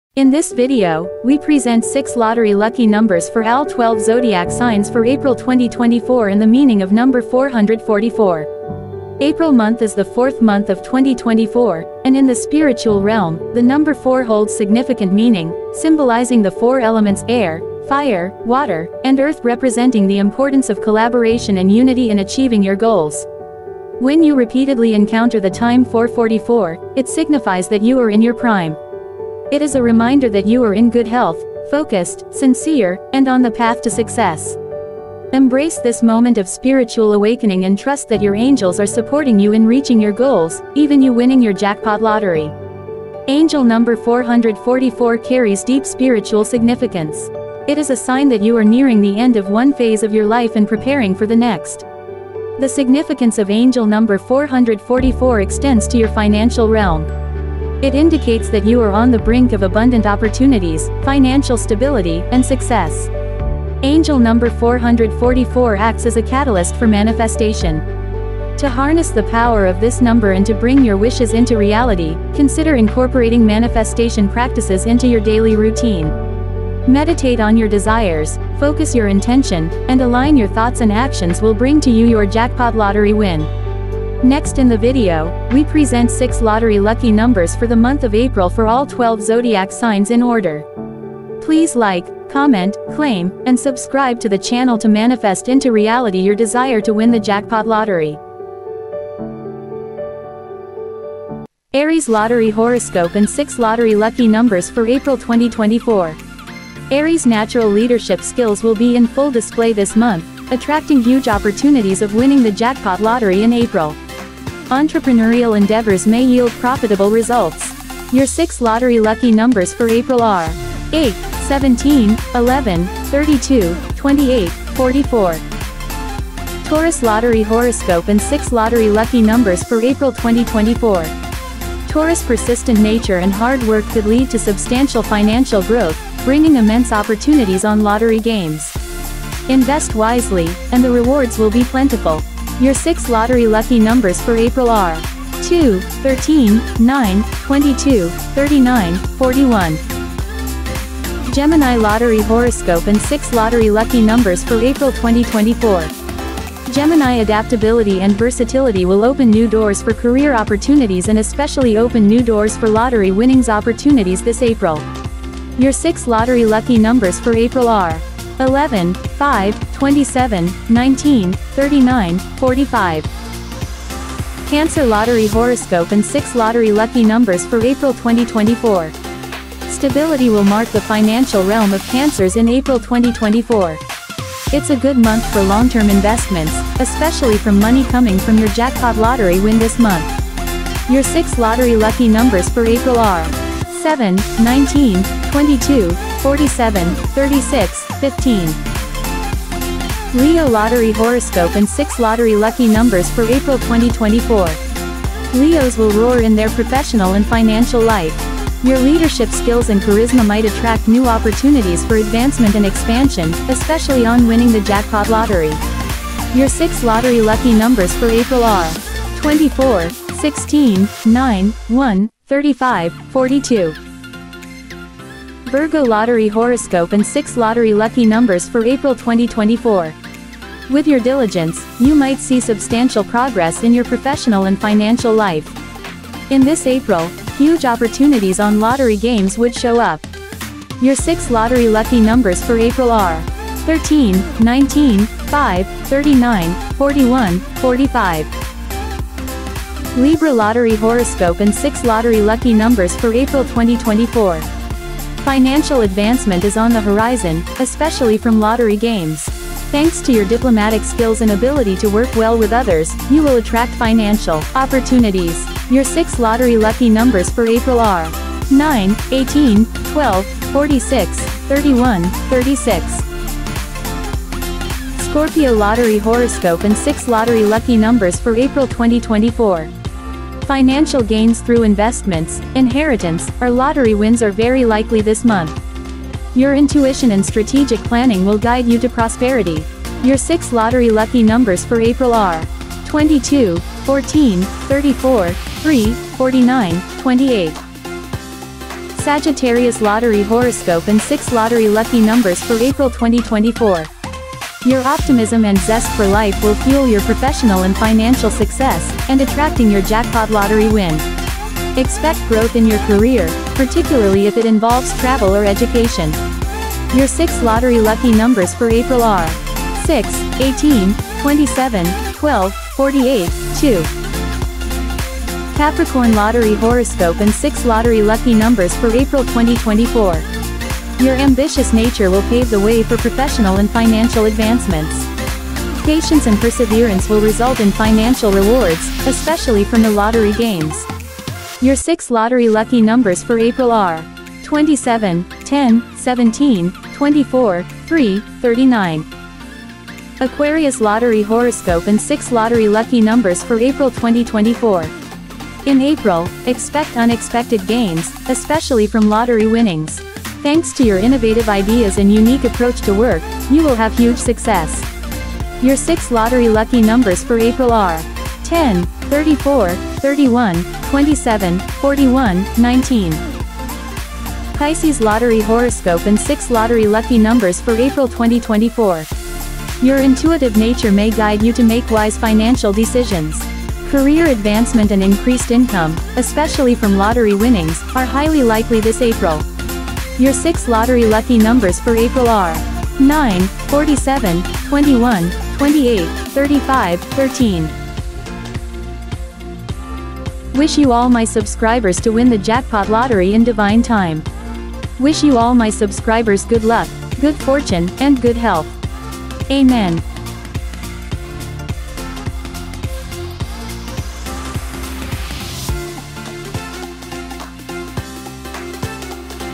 April 2024/6 Lottery Lucky Numbers ALL 12 Zodiac Signs/BONUS 444 Meaning#money #2024
In this video we present 6 Lottery LUCKY Numbers for all 12 Zodiac Signs for April 2024 and BONUS the meaning of number 444.
April month, is the fourth month of the year, and in spiritual realm, the number 4 holds significant meaning, symbolising the four elements ( air, fire, water, and earth) representing the importance of collaboration and unity in achieving your goals and manifesting your wishes.